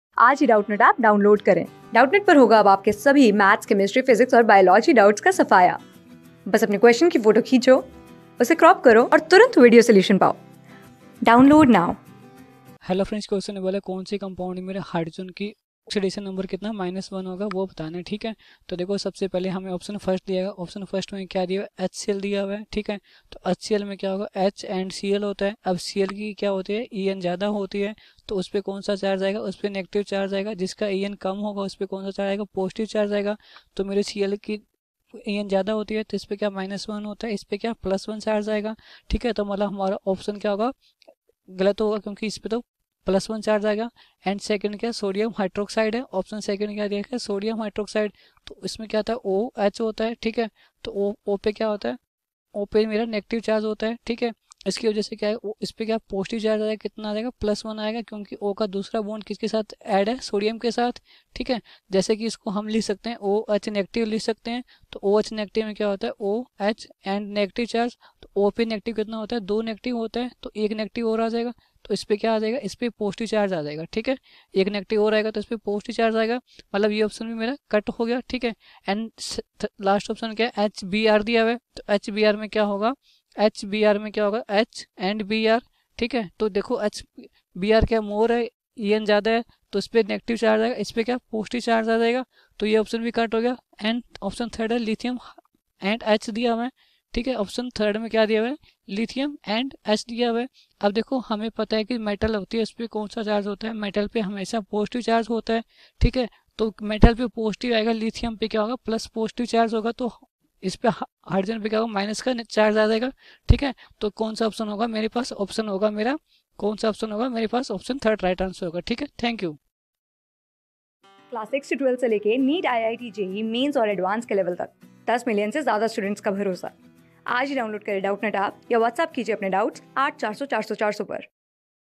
Which of the following compounds hydrogen has oxidation number of -1 ? | CLASS 12 | ATOMIC STRU...
Which of the following compounds hydrogen has oxidation number of -1 ?
Class: 12
Subject: CHEMISTRY
Chapter: ATOMIC STRUCTURE
Board:IIT JEE

👉 Have Any Query? Ask Us.